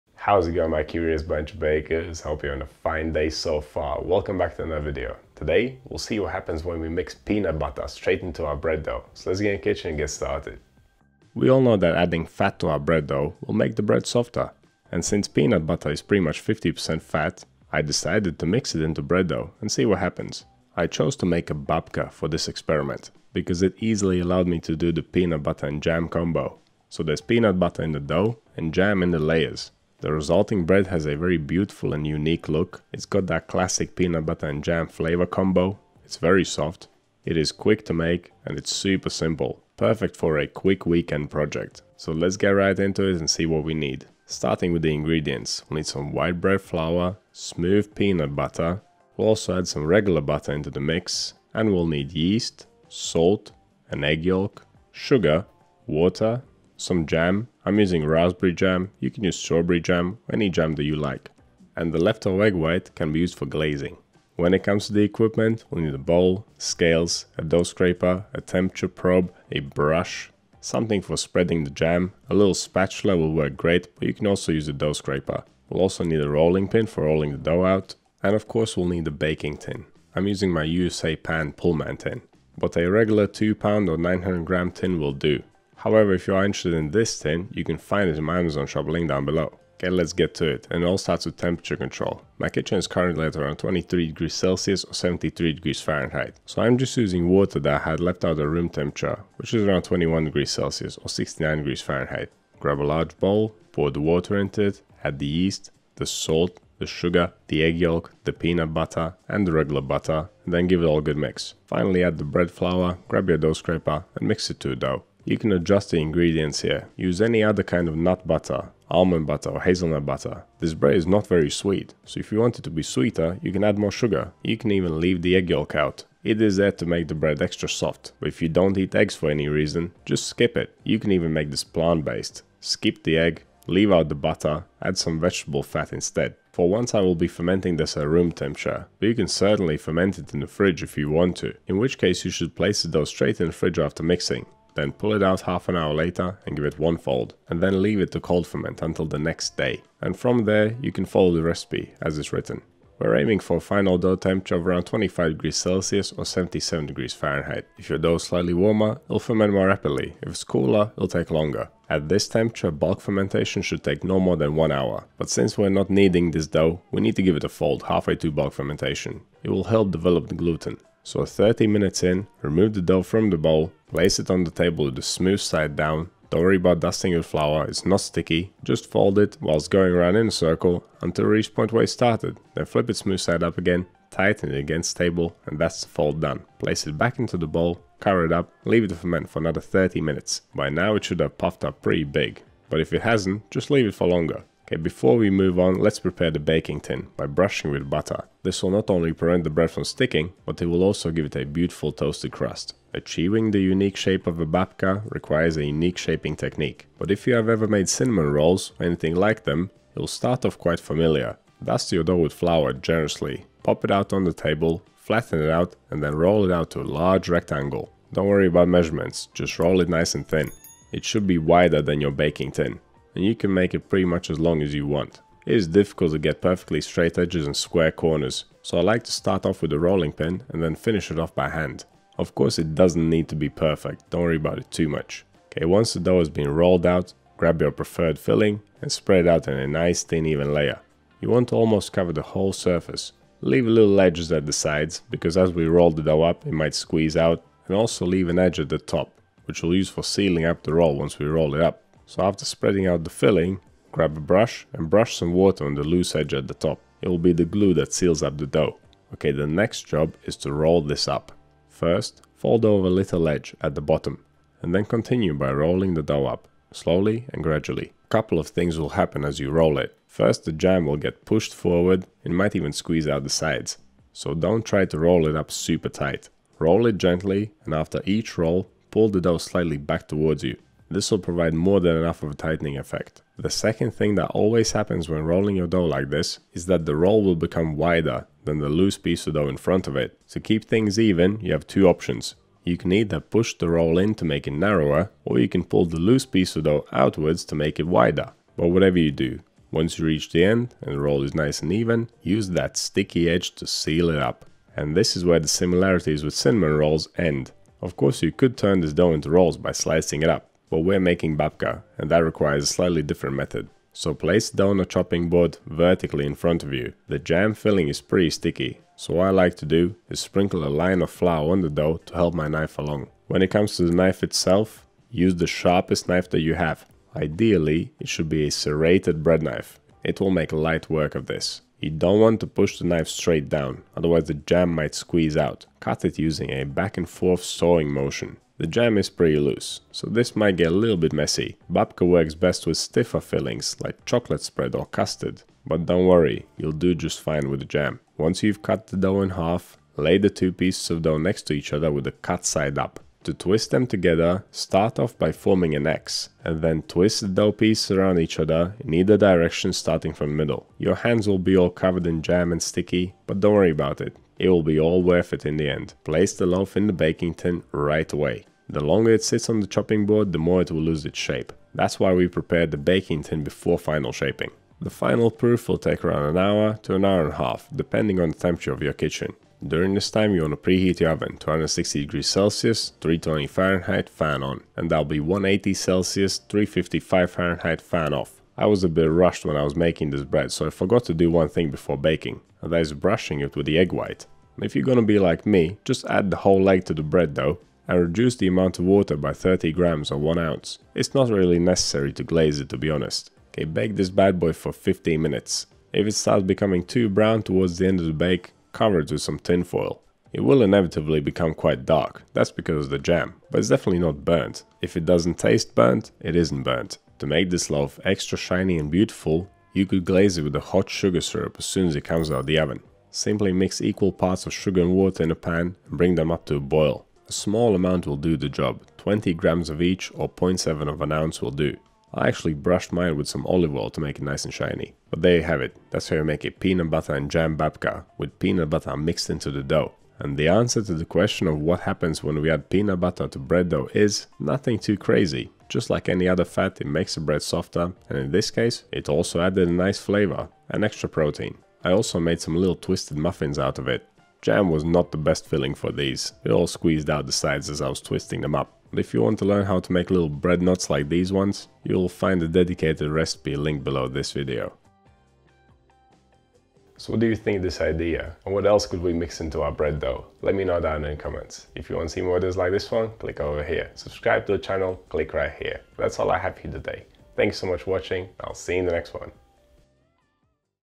How to Make a Peanut Butter & Jam Babka
The unique design of a babka is achieved by filling the dough, rolling it up, cutting in half, and finally twisting back together. The roll is made the same way that you’d make cinnamon rolls, but instead of cutting it into slices it is cut lengthways. Once sliced in half it reveals beautiful layers of dough and filling which make for a fantastic looking loaf in the end.

🥐 Learn all about bread making here ⤵️